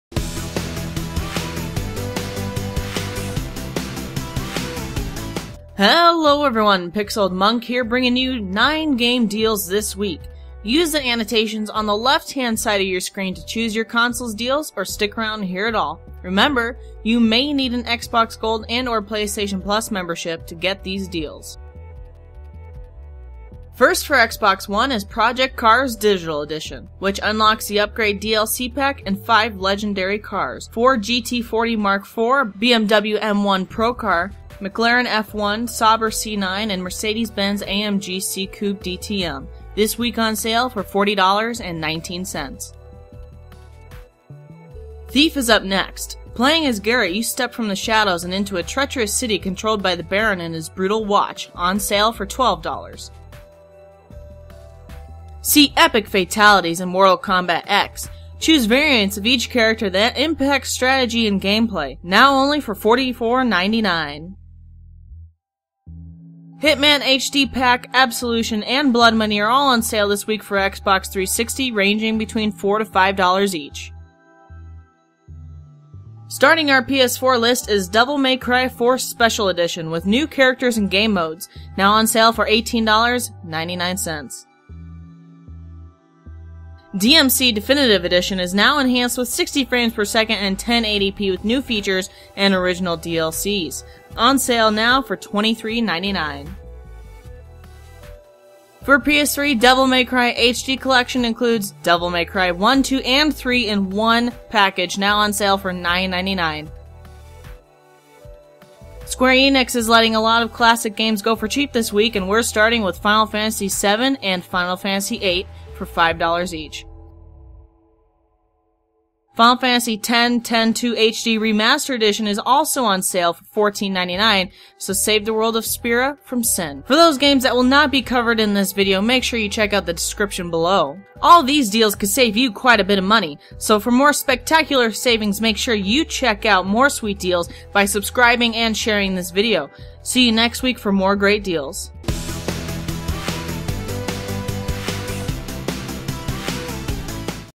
Game Deals of the Week - August 11-17 - For Xbox and PlayStation
Game deals for August 11-17, 2015 for your Xbox and PlayStation!!
Using your Xbox Live Gold membership and/or PlayStation Plus membership you can access great deals on games using your Xbox Live Deals with Gold or PlayStation Plus Sale.

This week includes:
Project Cars: Digital Edition
Thief
Mortal Kombat X
Hitman HD Pack, Absolution and Blood Money
Devil May Cry 4 Special Edition
Devil May Cry Definitive Edition
Devil May Cry, DMC 2 and DMC 3